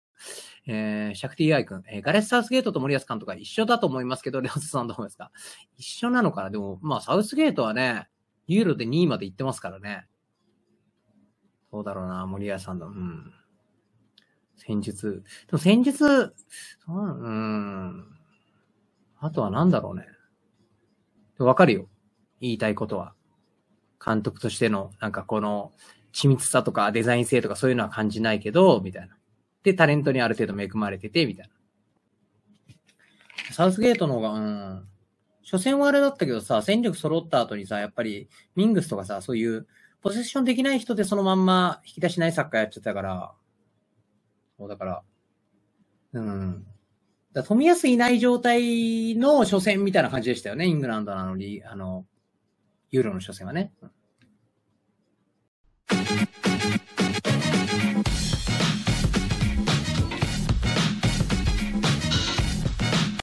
ガレスサウスゲートと森保監督は一緒だと思いますけど笑、レオザさんはどう思いますか？【レオザ切り抜き】
※このチャンネルはLeo the football TVさんから許可をもらって運営しています